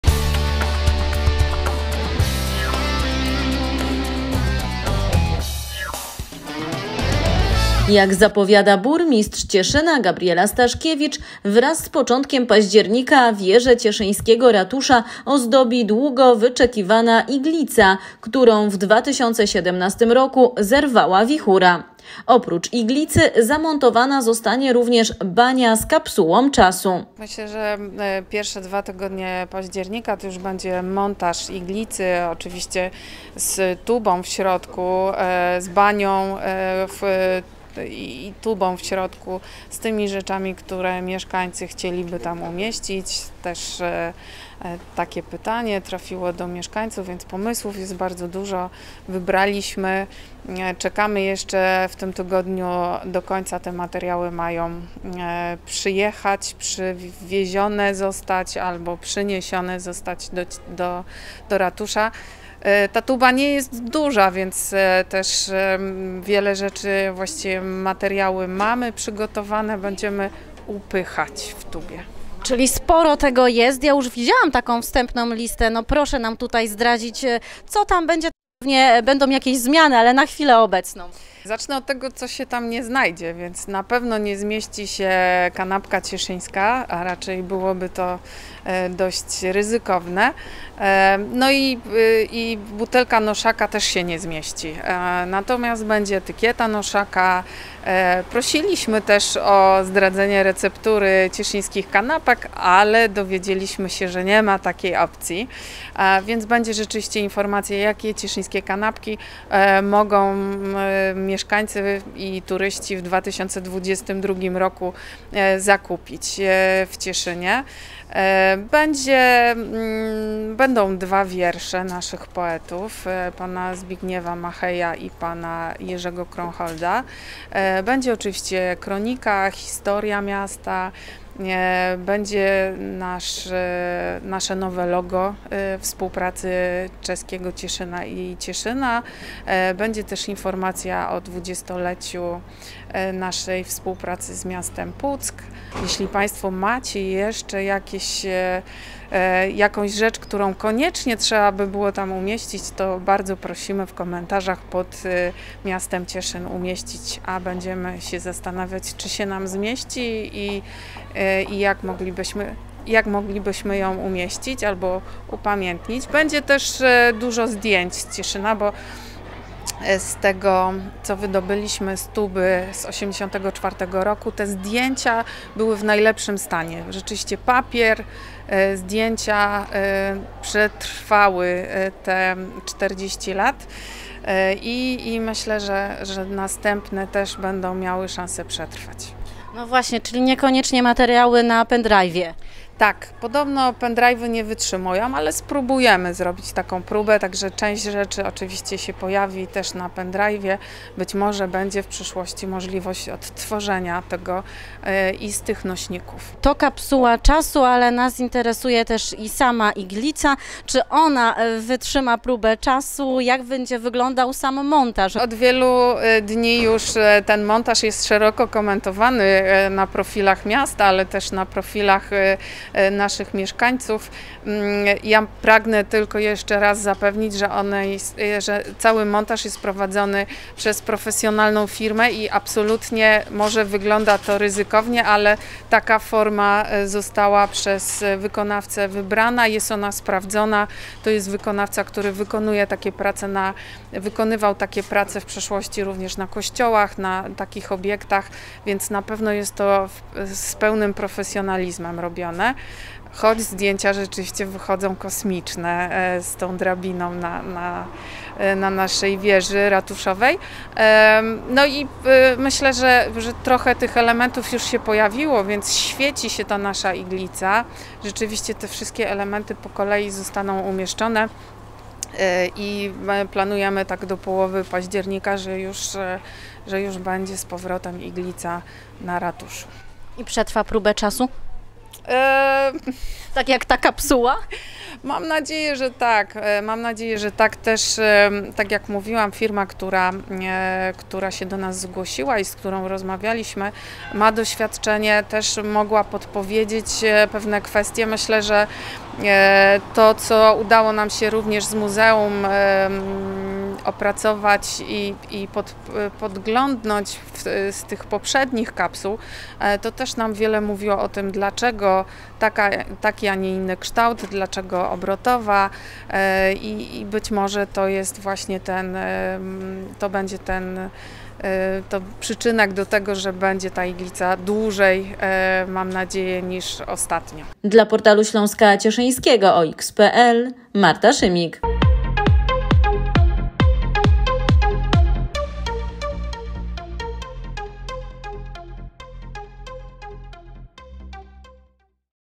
Cieszyn: Wielki powrót iglicy
W październiku na wieżę ratuszową wreszcie wróci iglica. Jak informuje cieszyński magistrat prace są w toku. Oprócz iglicy zamontowana zostanie również kapsuła czasu.